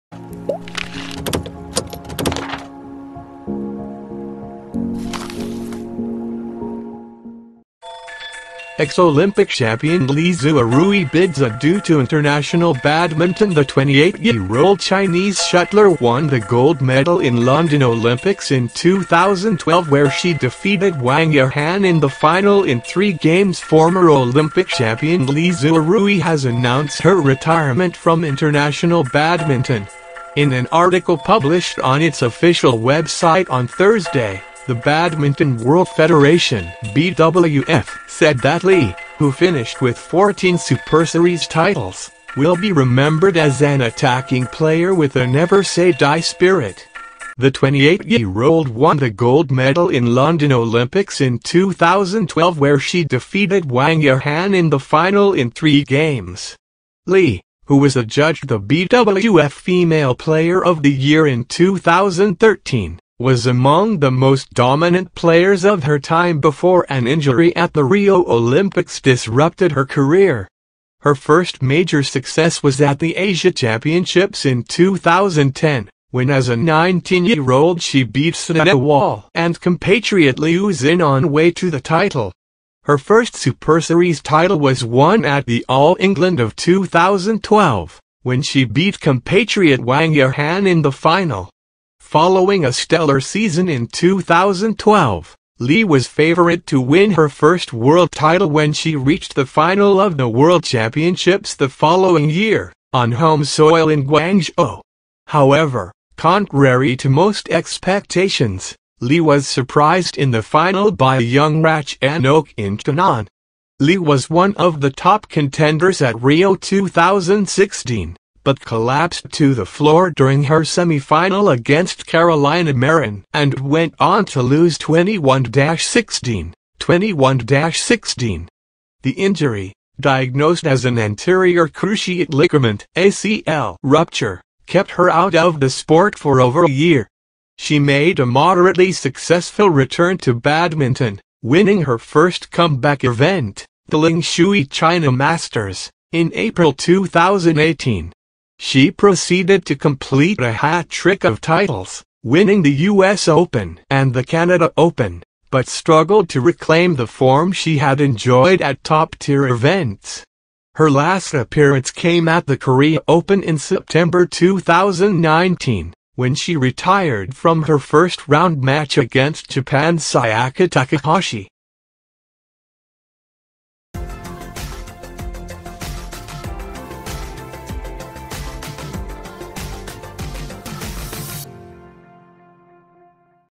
Ex-Olympic champion Li Xuerui bids adieu to int’l badminton
Flash
Channel specialized in publishing the most important news
"Entertainment - Sports - Health - Science / Technology"